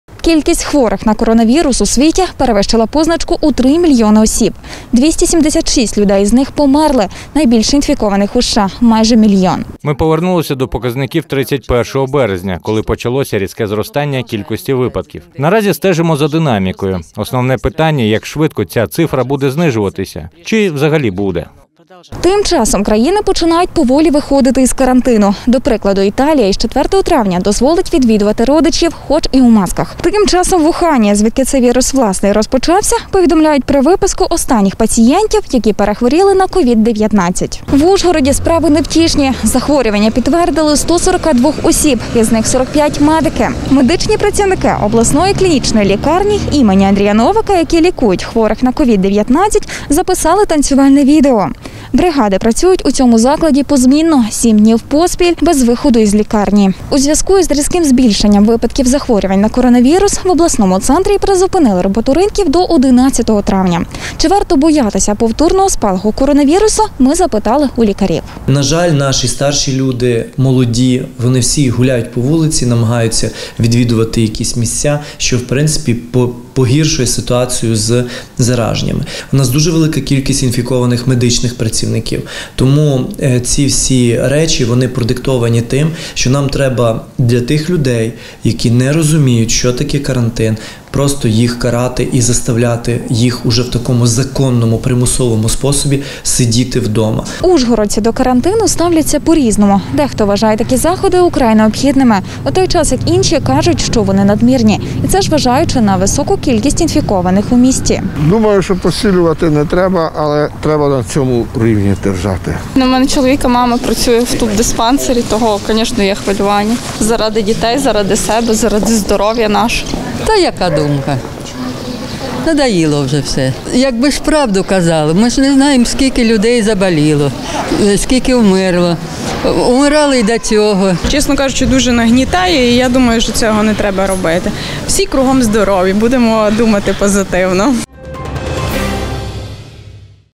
Місто на карантині: у 142 ужгородців підтверджено COVID-19
Кількість хворих на коронавірус у світі перевищила позначку у 3 млн осіб. Від ускладнень померли 206 тисяч людей. Деякі країни поволі починають виходити з карантину. В Україні ж за минулу добу зафіксували 392 нові випадки зараження коронавірусом. В Ужгороді справи невтішні: захворювання підтвердили у 142 хворих. Серед них 45 медиків. У той же час чимало ужгородців підтримують послаблення карантину, а дехто і зовсім сумнівається в існуванні захворювання.